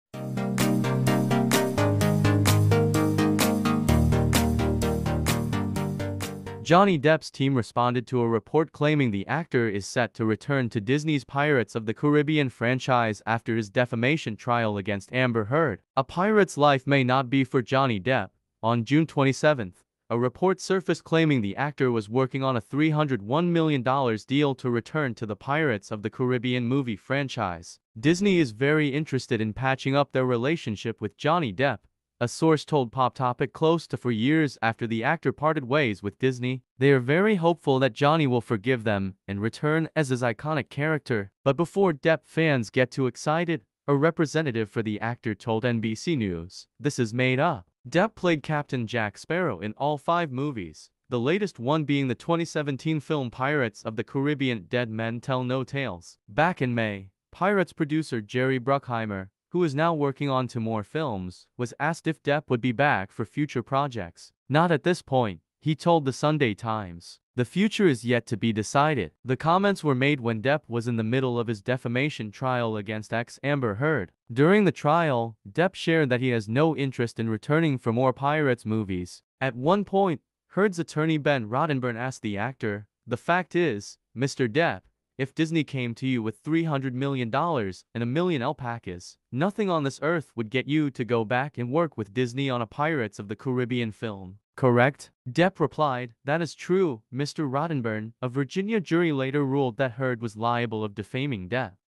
Disney offered Johnny Depp $ 300 million to return to Pirates of the Caribbean Johnny depp's answer?
Johnny Depp’s team responded to a report claiming the actor is set to return to Disney's Pirates of the Caribbean franchise after his defamation trial against Amber Heard.